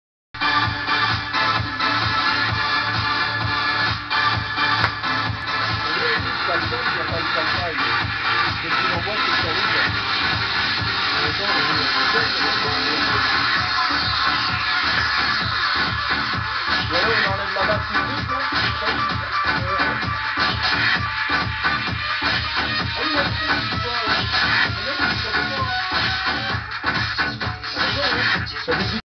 Mix Live Digan Hiaumet
Un mix live 1H de Digan Hiaumet sur Radio RAM !